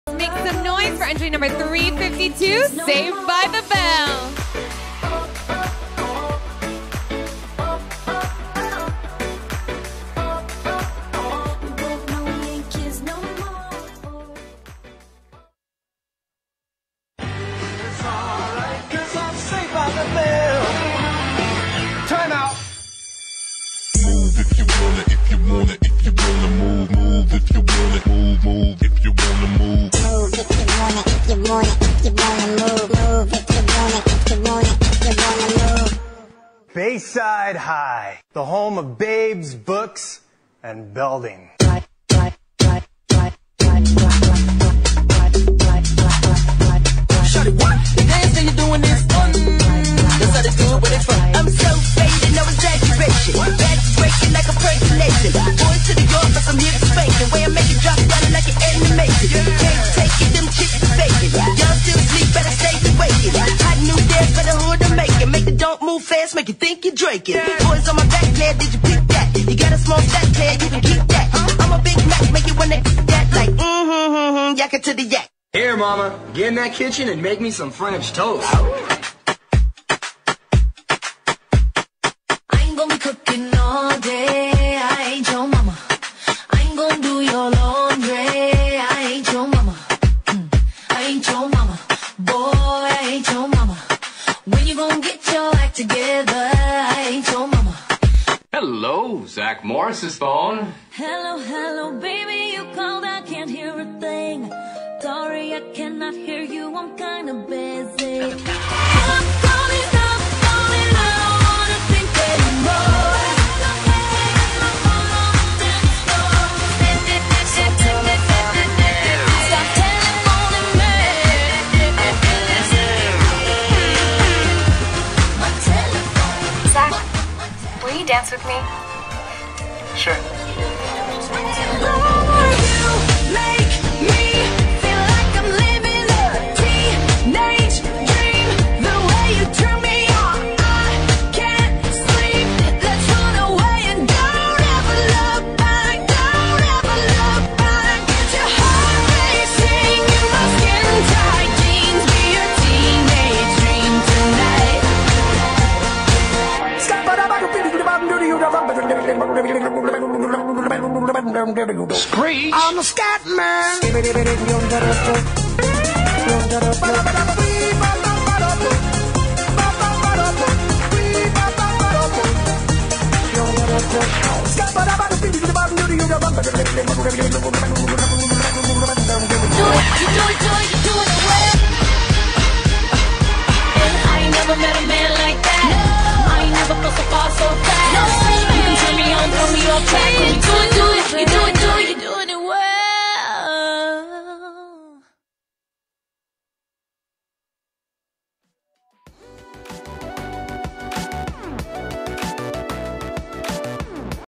2017 IDA Nominee (Hip Hop) - Toledo, OH - Z Company
This performance will be in the running to represent Groove Dance Competition at the 2017 Industry Dance Awards and Cancer Benefit Show.

"Saved by The Bell" performed in  Toledo, OH by Z Company.

Groovecompetition.com
IndustryDanceAwards.com